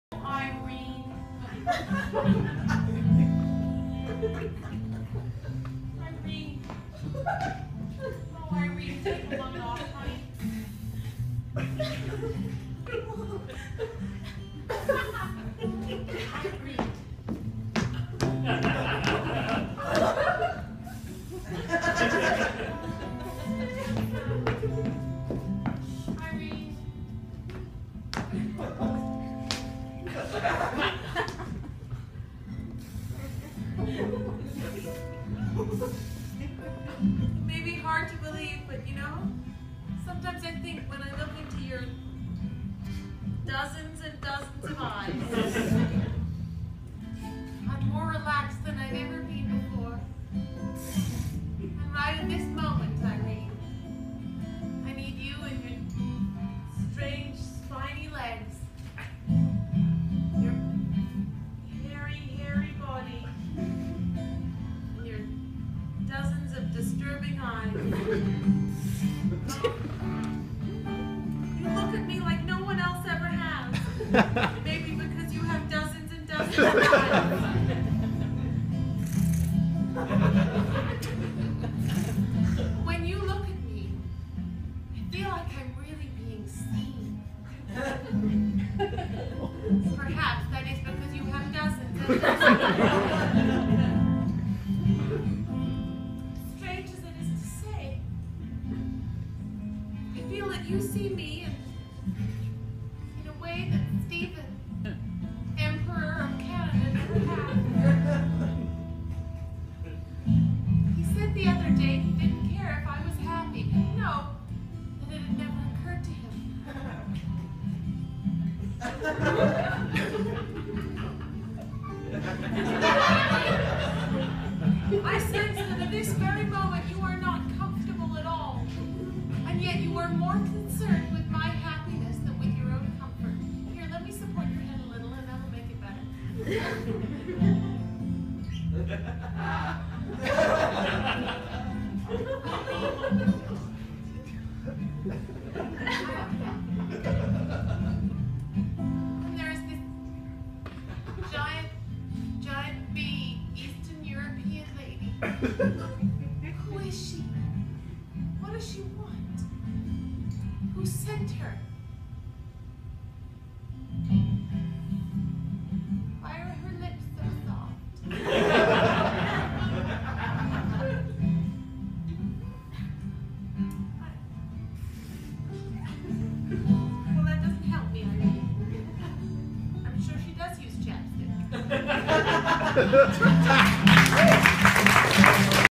Laureen Harper and Irene, the stress tarantula.
From Die-Nasty's 23rd annual soapathon, House of Soap, September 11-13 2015, at the Backstage Theatre in Edmonton, Alberta.